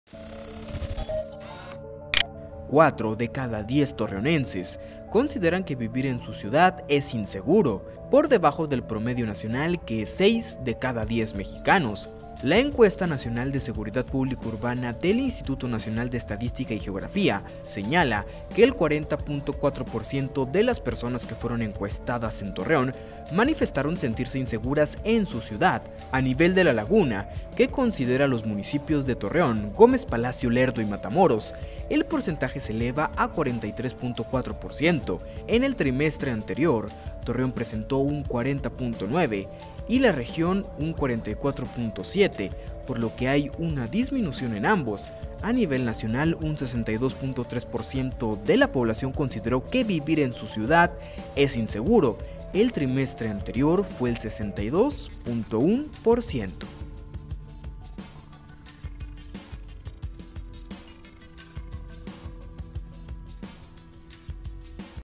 Cuatro de cada diez ciudadanos consideran que es inseguro vivir en Torreón
Cuatro de cada 10 torreonenses consideran que vivir en su ciudad es inseguro, por debajo del promedio nacional, que es de 6 de cada 10 mexicanos.